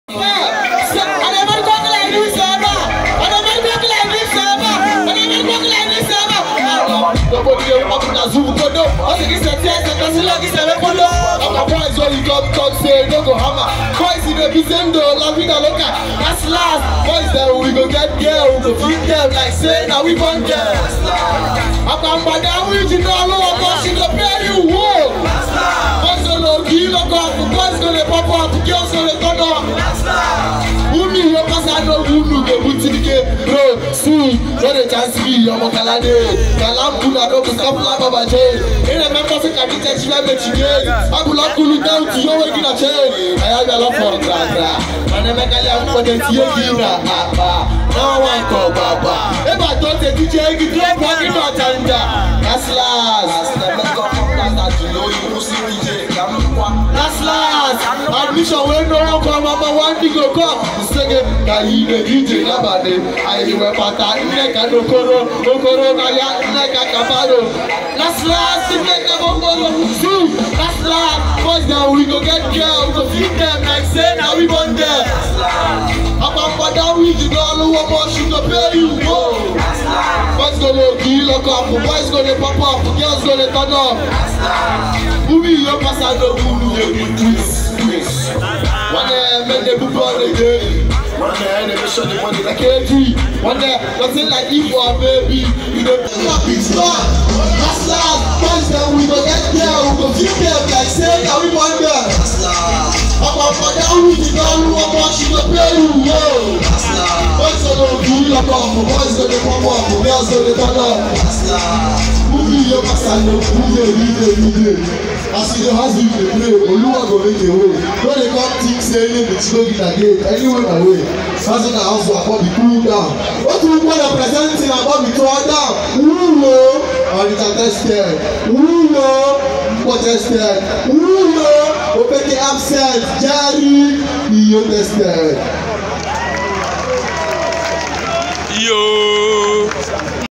JERIQ DOING HIS THING AT THE IKOLO CONCERT ALONGSIDE ZORO,SLOWDOG AND RUNTOWN AT THE IKOLO CONCERT
JERIQ AT THE IKOLO CONCERT